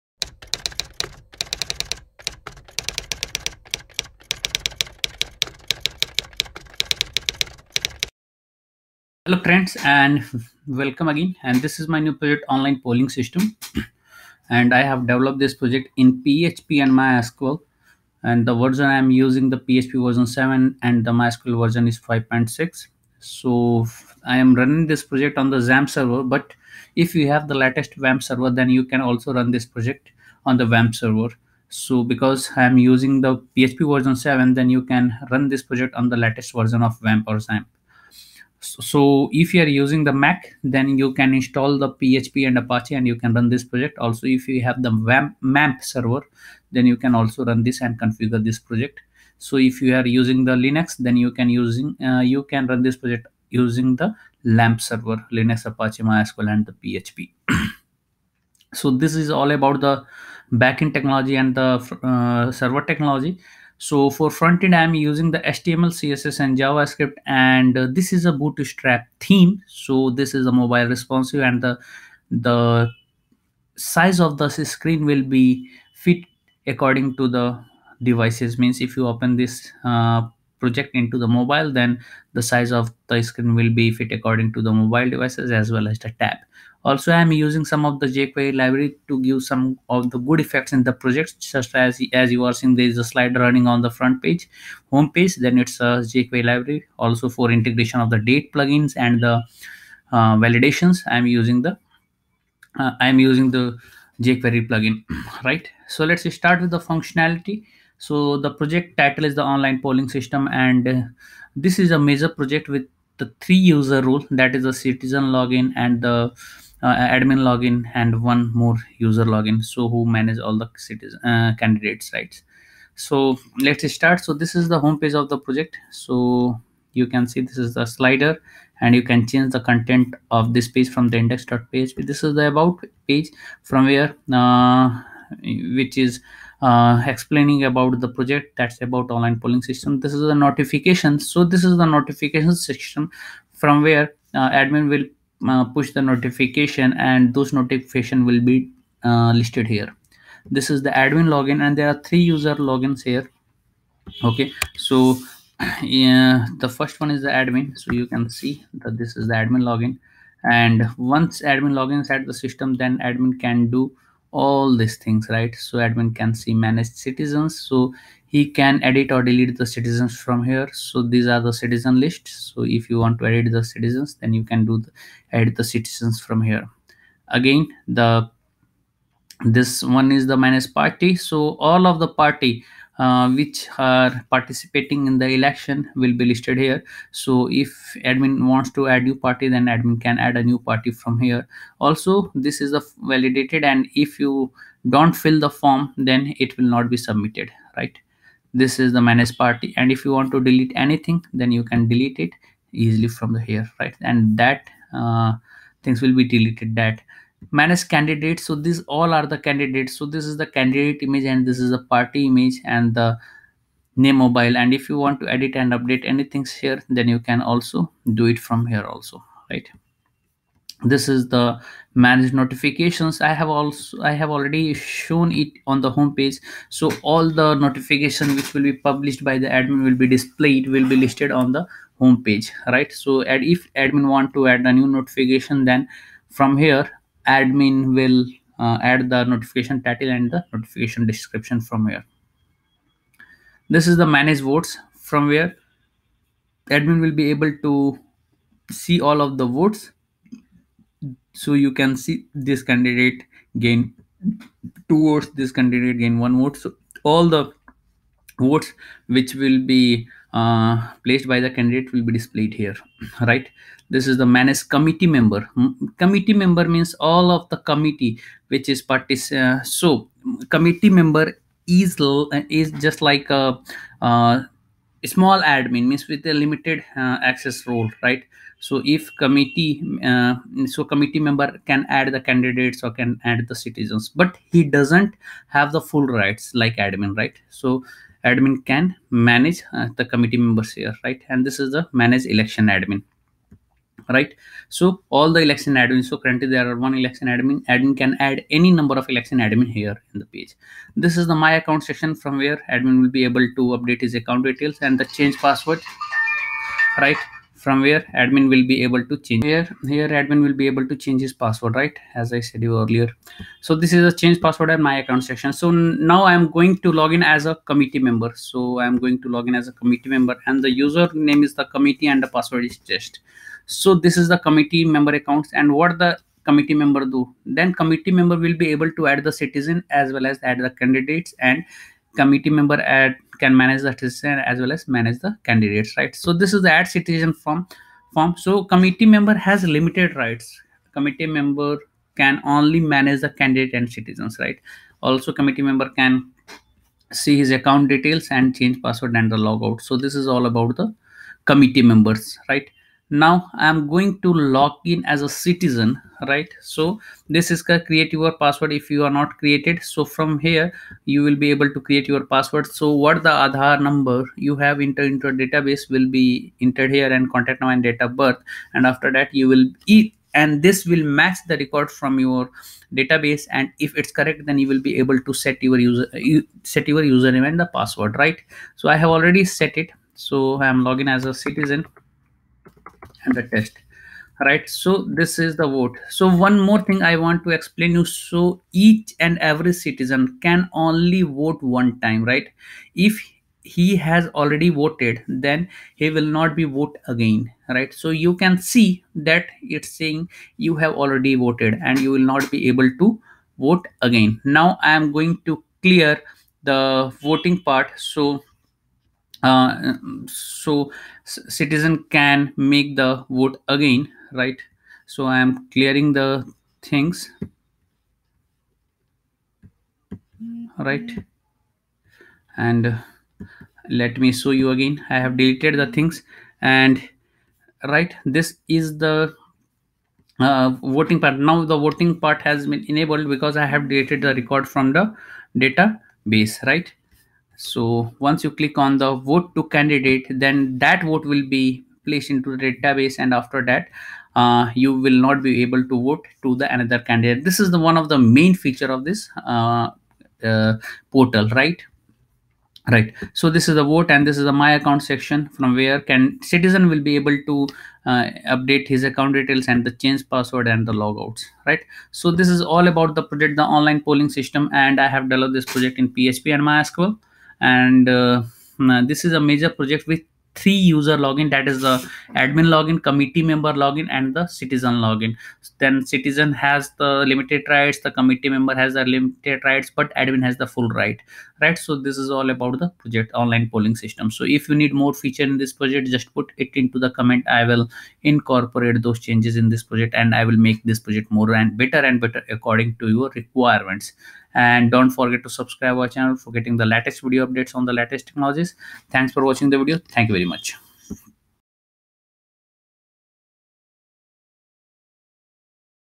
Online Polling System | PHP and MySQL Project Source Code | PHP MySQL CRUD Project
Online Polling System is a PHP MySQL micro service project where all APIs we have developed in PHP MySQL and all UI Online Polling System  we have implemented in PHP.

Online Polling System PHP and MySQL Project Tutorial
Online Polling System Download